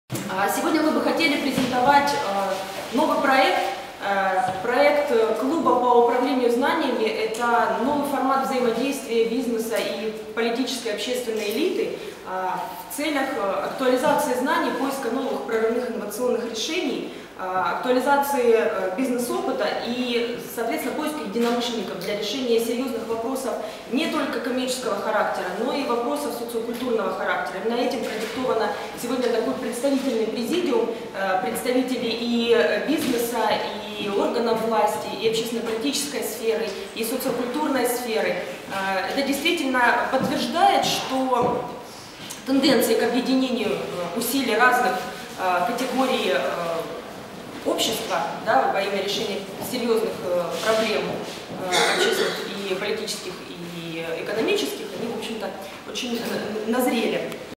Каким будет проект «MegaPROFit»? Марина Демченко.1
Старту ростовского проекта «MegaPROFit» его организаторы 16 января посвятили пресс-конференцию в пресс-центре «МедиаС».